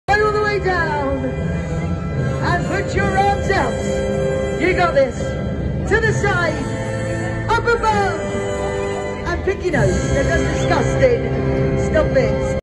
Starting the Magic Show at Stanwell in Christmas!
This is a video of me starting my show at a Christmas event